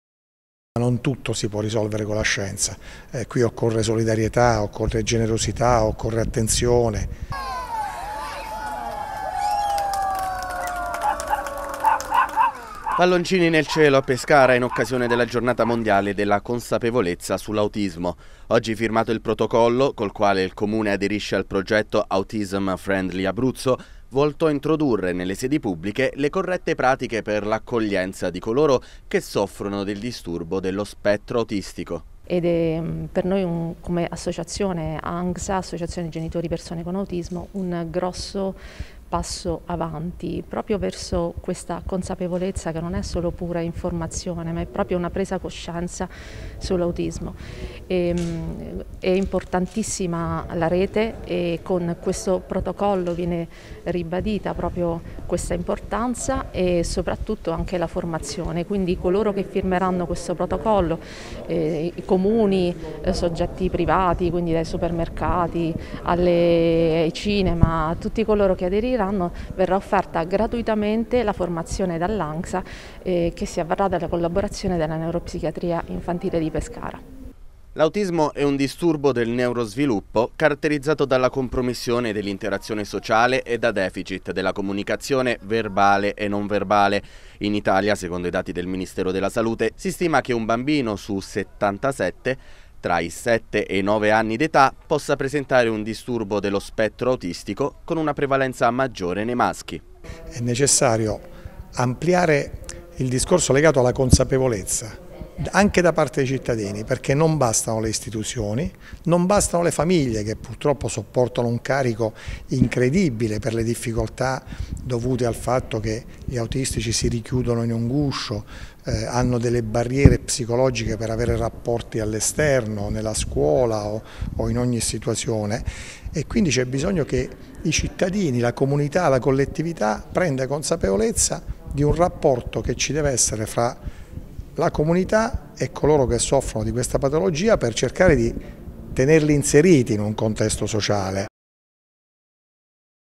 Pescara. Il Comune aderisce al progetto "Autism Friendly Abruzzo"
Firmato un protocollo oggi a Pescara, in occasione della giornata mondiale dell'autismo, col quale il Comune adotta le corrette pratiche nei confronti di chi soffre del disturbo dello spettro autistico. L'Angsa, l'associazione genitori persone autistiche, si occuperà della formazione